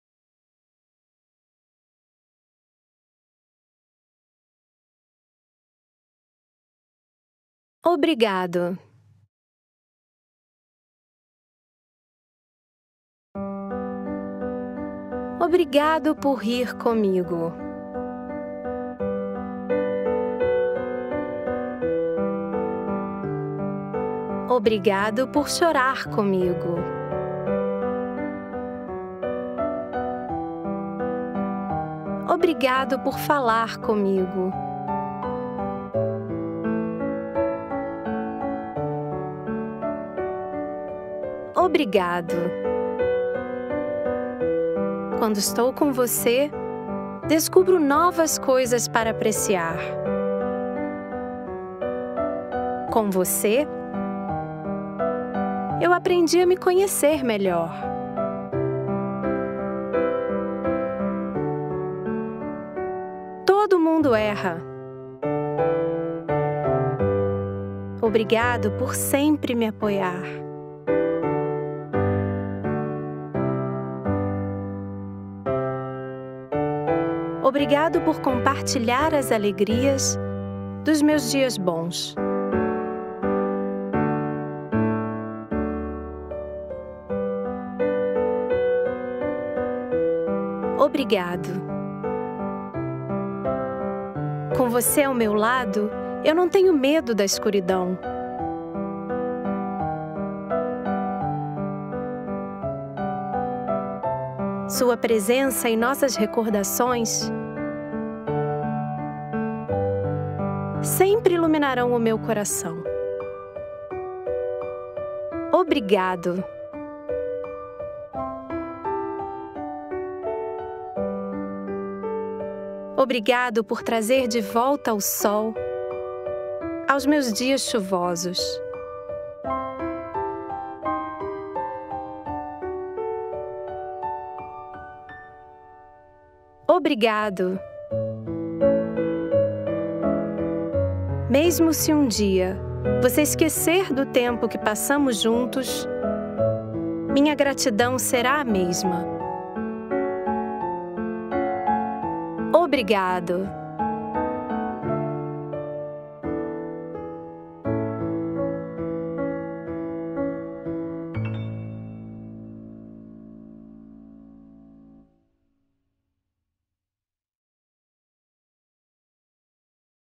Contos do Kirby Leitura acompanhada 7： Obrigado
Temos o prazer de apresentar um vídeo de leitura acompanhada da série de livros ilustrados "Contos do Kirby", um conjunto de histórias emocionantes para alegrar o seu dia. São sete vídeos de leitura acompanhada no total.

Esta é a história perfeita para  as pessoas mais importantes da sua vida. Nem sempre lembramos de expressar nossa gratidão, então Kirby e seus amigos estão aqui para nos ajudar partilhar nossos sentimentos. Acompanhe a história enquanto os personagens falam "obrigado" em diversas situações na sossegada Dream Land.

Narração em português da adaptação de Kirby Time: Thank You (Livro 5), publicado por Shogakukan.
Ilustrações de RURUTEA
Texto de Asami Taniguchi

■ Página oficial do Kirby

■ Página oficial de Kirby's Return to Dream Land Deluxe

■ Página oficial de Kirby and the Forgotten Land
■ Página oficial de Kirby's Dream Buffet